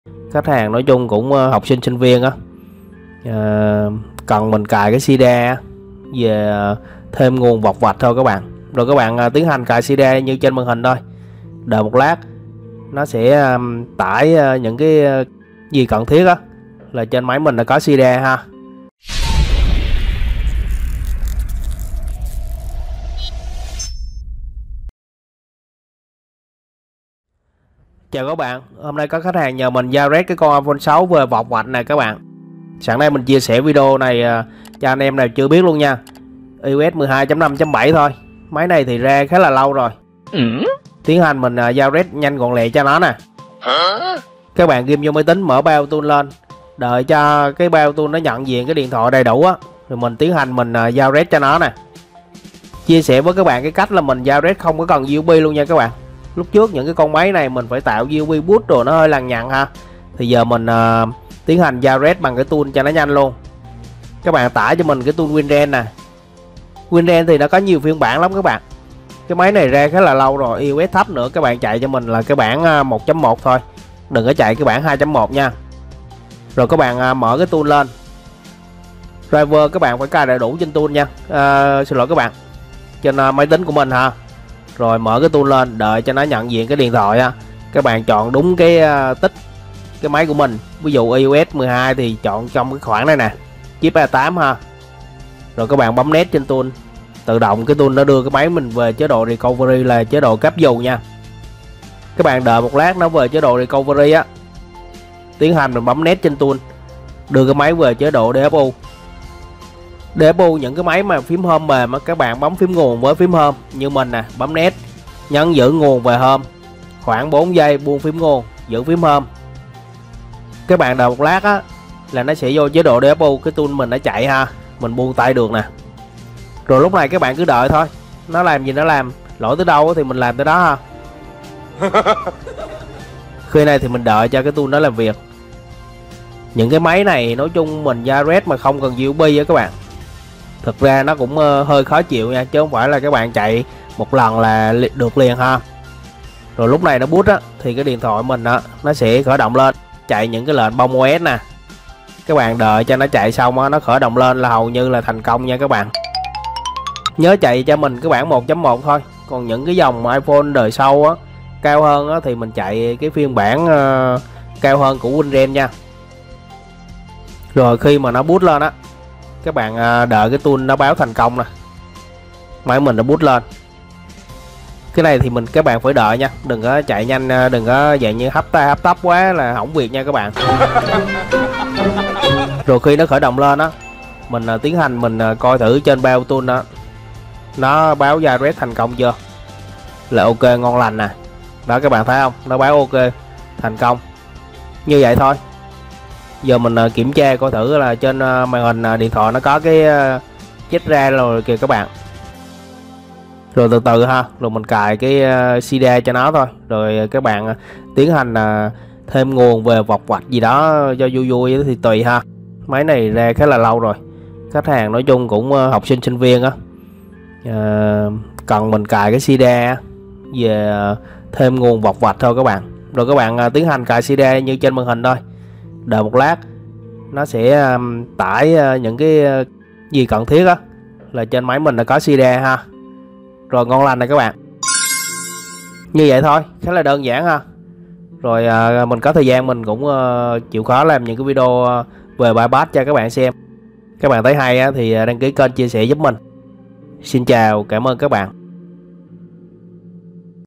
Cách Jailbreak/Cài Cydia cho iPhone 6/6 Plus iOS 12.5.7 không cần USB
Hướng dẫn các bạn Cách Jaibreak iPhone 6/6+ iOS 12.5.7 bằng tool WinRa1n nhanh chóng, không cần Boot USB trên máy tính Windows